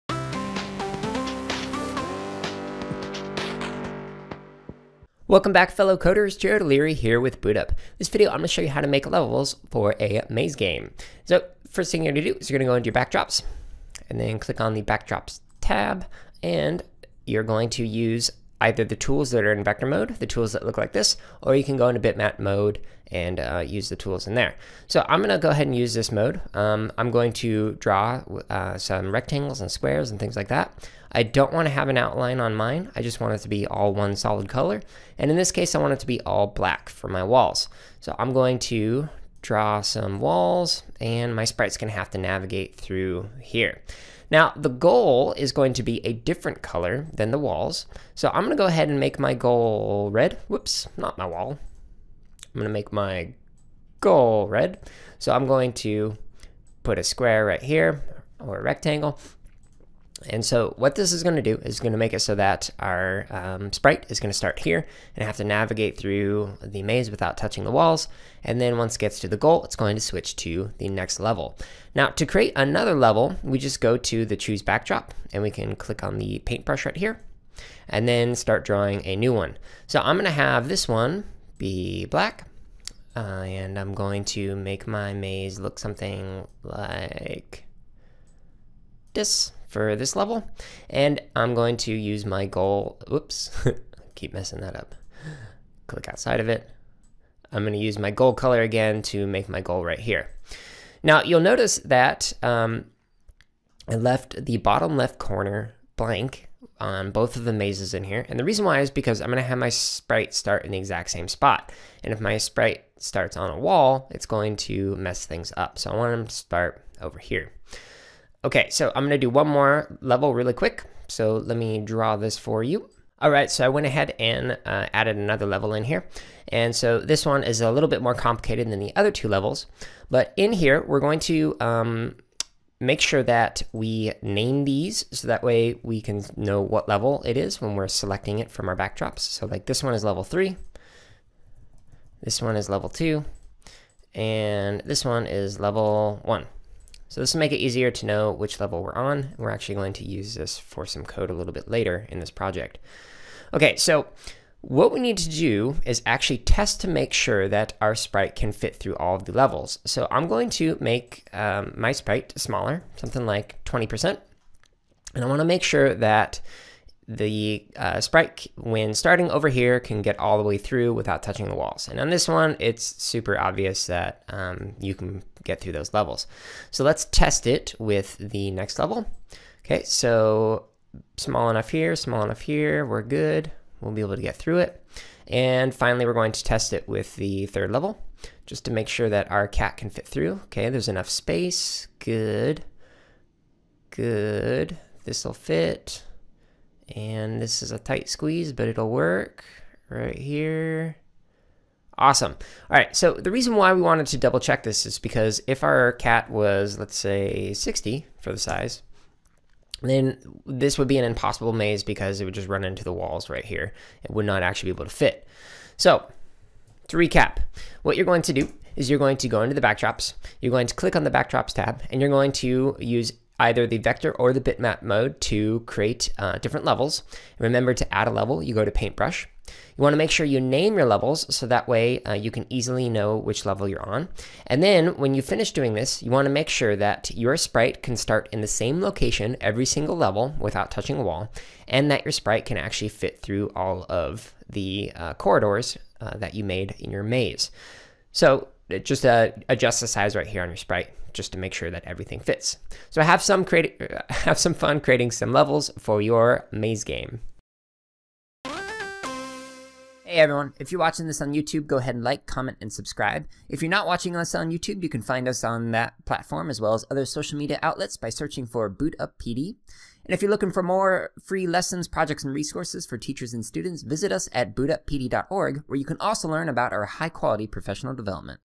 An Amazing Maze Game - Create Levels
Coders create a player controlled maze game with multiple, custom levels. The purpose of this project is to introduce conditional statements (if blocks) to create player controls, while reinforcing how to use the image editor to design mazes.

Follow us for our latest updates!